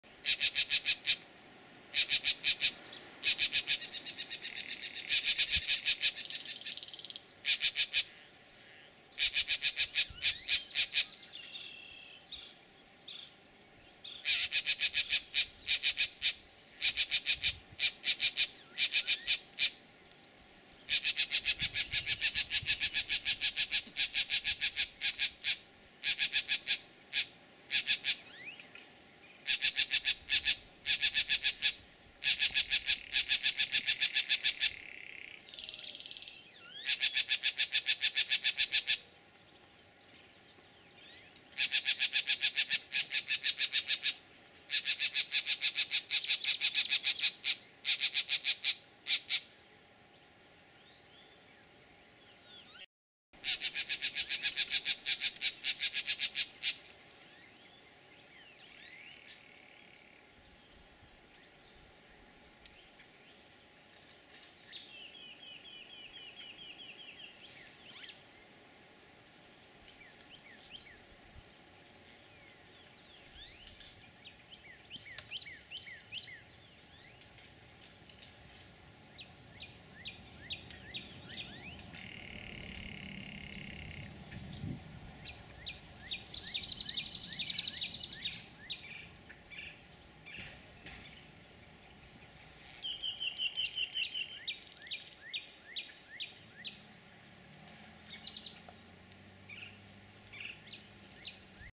Bullsnake. Laguna Atascosa NWR
Recorded at Laguna Atascosa National Wildlife Refuge. Los Fresnos Texas. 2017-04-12
Green Jays sounding the "snake alarm".
Pituophis catenifer ssp. sayi,
?  (Spanish)